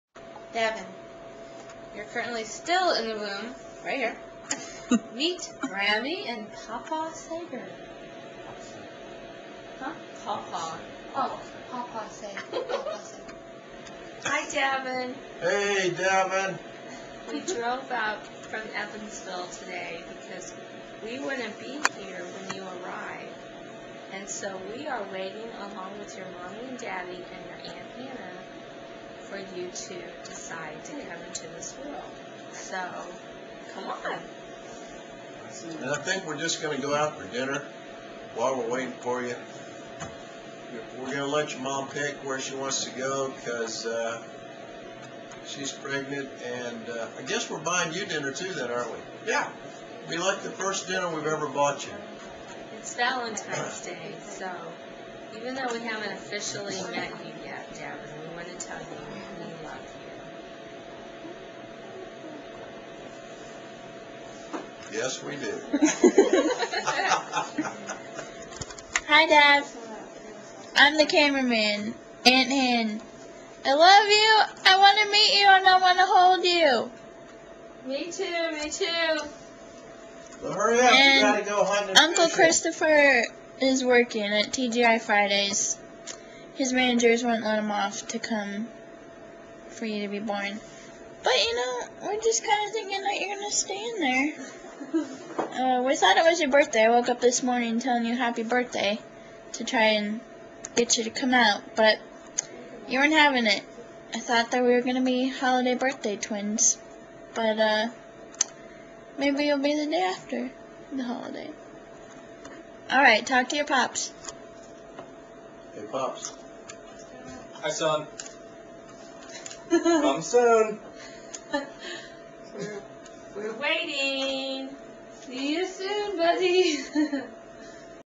Message to Davin @ 6 Days Overdue
We started recording messages to Davin when he was still in Misha's womb.  This is second one at 6 days past his due date.  This time Paw Paw Seg, Grammy, & Aunt Han chime in.